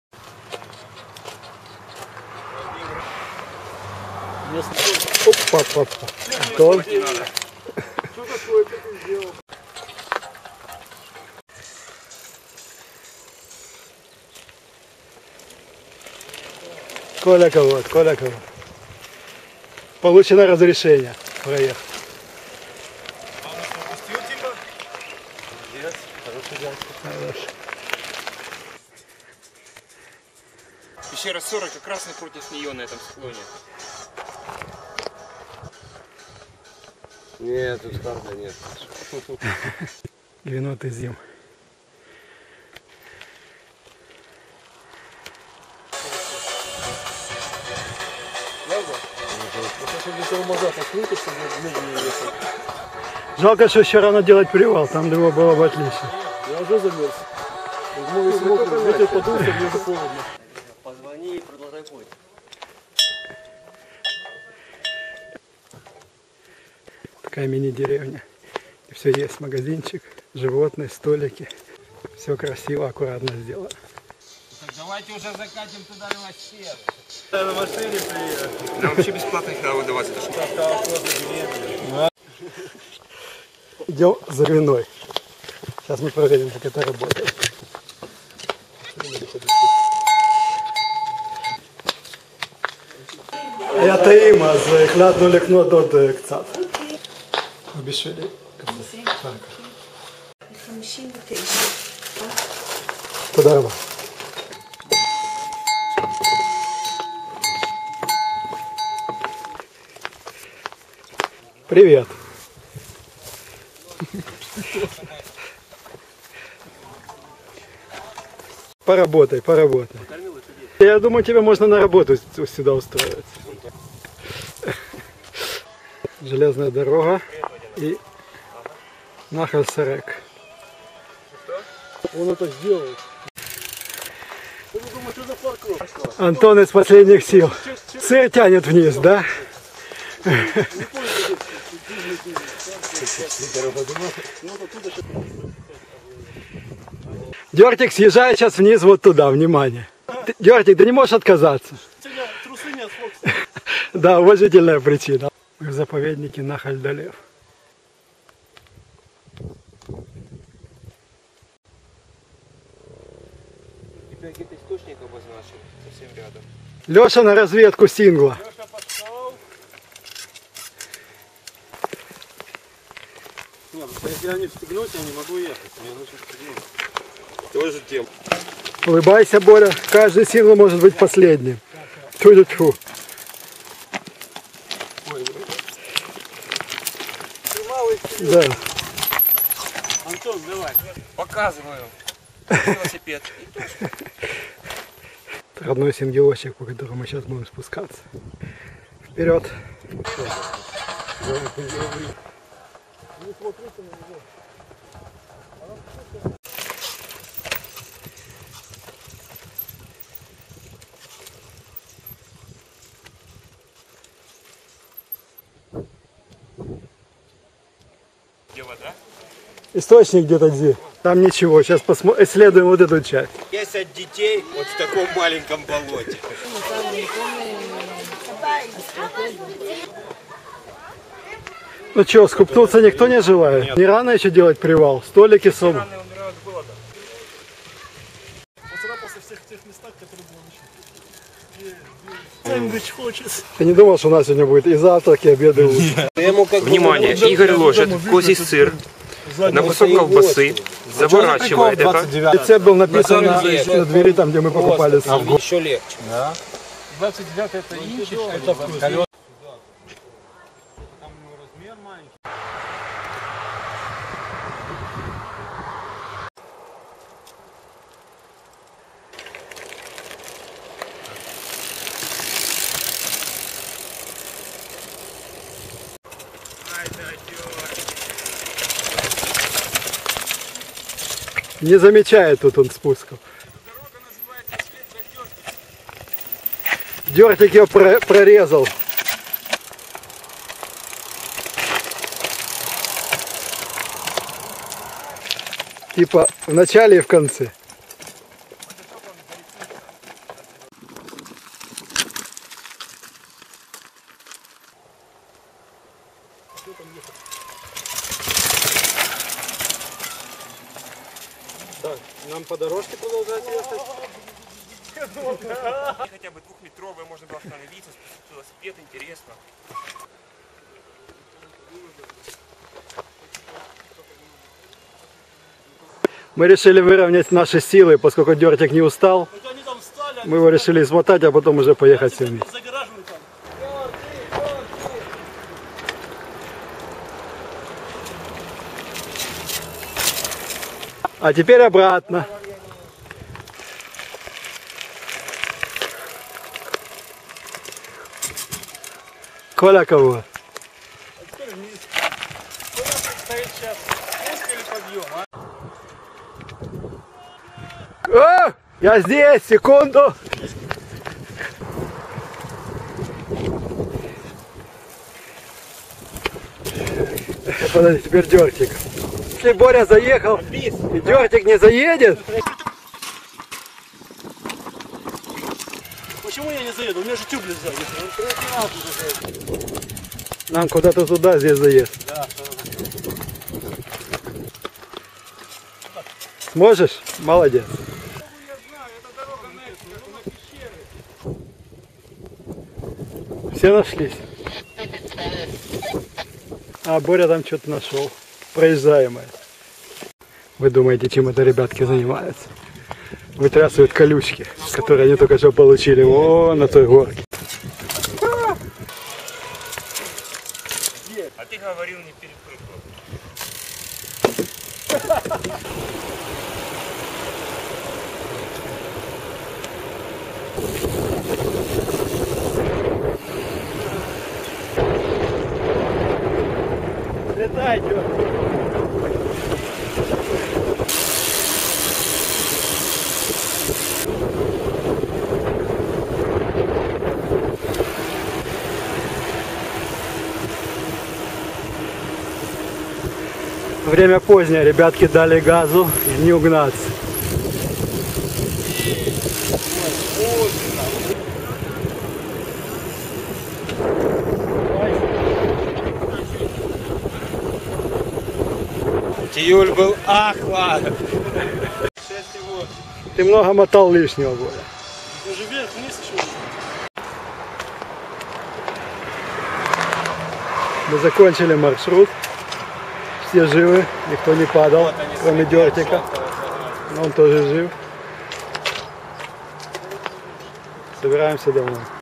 Mountain biking. Israel. Beit Shemesh - Reserve Nahal Dolev.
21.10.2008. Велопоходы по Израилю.
Бейт Шемеш - заповедник Нахаль Долев.